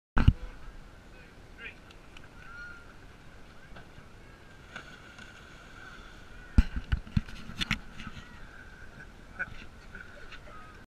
Coniston pier jump
Jumping from the pier into Coniston water 8/8/2020